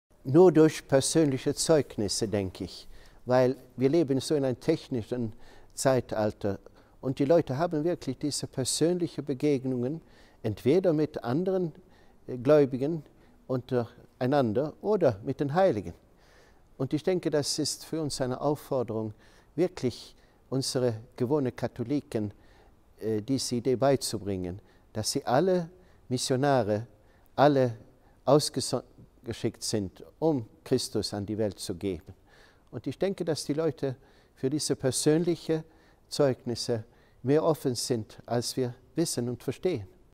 "Werde Glaubensstifter!" Kardinal Arborelius zum diesjährigen Leitwort der Diaspora-Aktion
Kardinal Arborelius aus Schweden haben wir mit Blick auf das Leitwort unserer aktuellen Diaspora-Aktion "Werde Glaubensstifter!" die Frage gestellt, wie wir heute Menschen für den Glauben begeistern können.